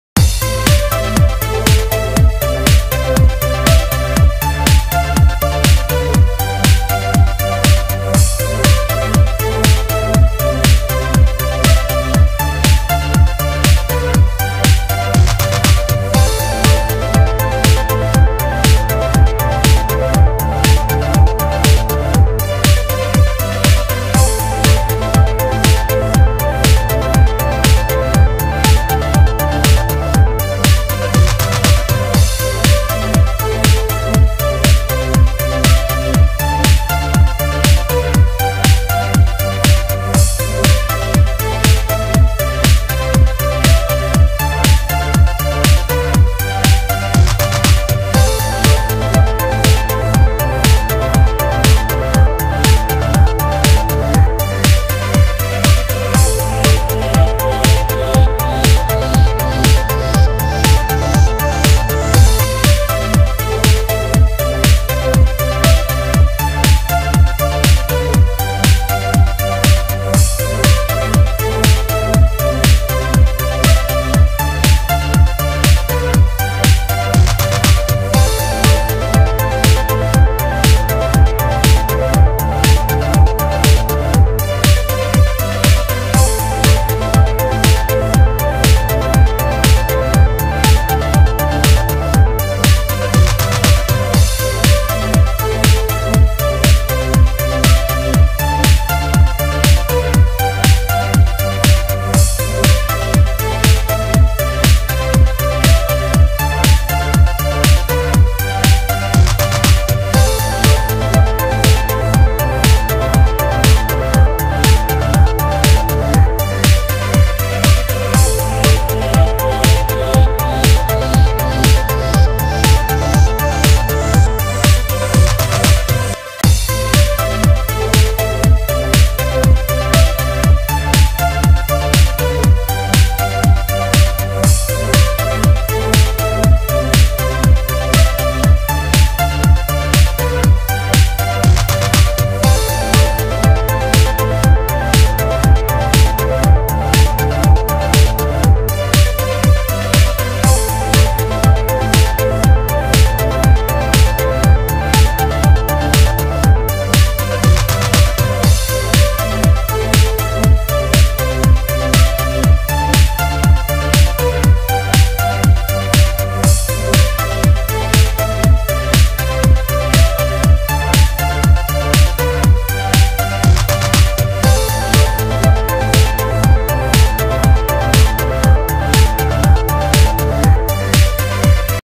FUDGEE BARR ICE CREAM CAKE.. no bake needed...
make ur own version of cake..good thing is no need oven or no need to bake...